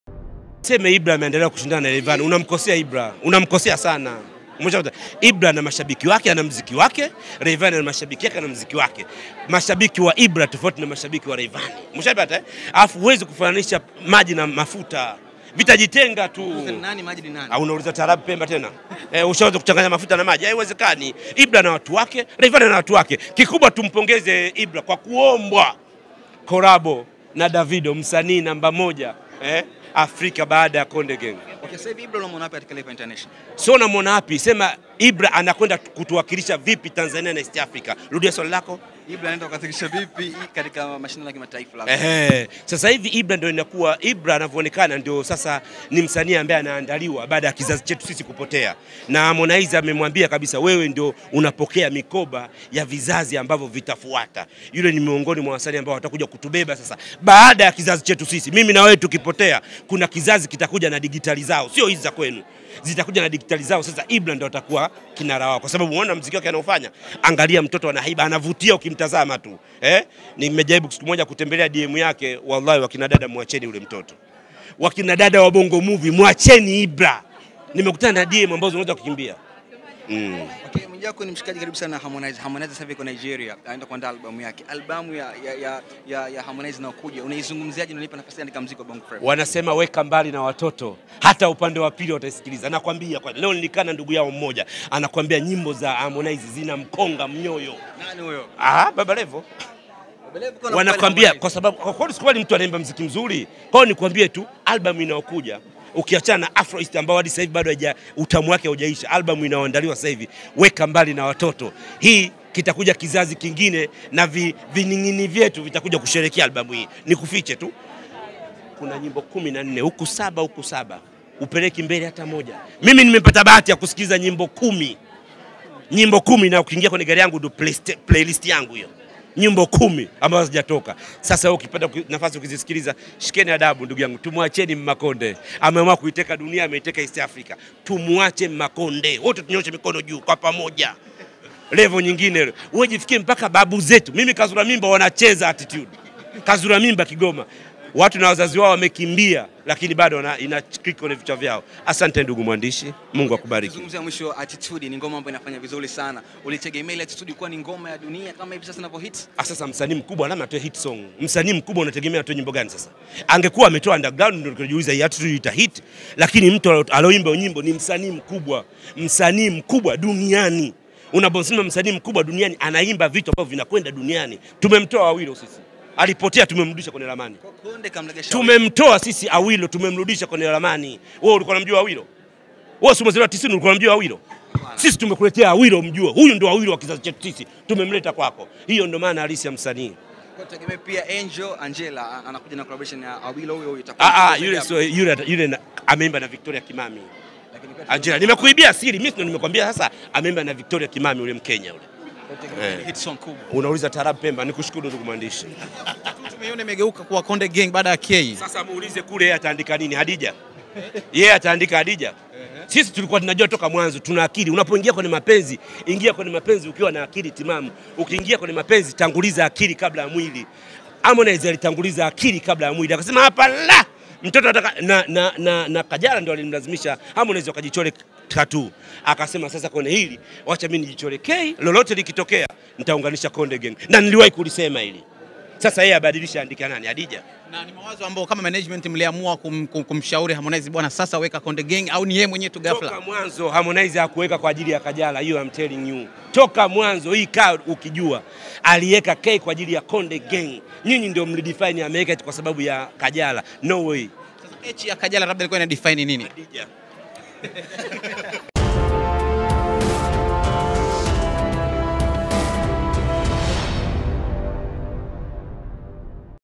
Kajala alimforce Harmonize achole tattoo sio kwamba walikuwa wanapendana
MWIJAKU_KAJALA TATOO ACHORE HADIJA_HARMONIZE ALIKUA HAMPENDI KAJALA ALIMFORCE WACHORE TATOO.mp4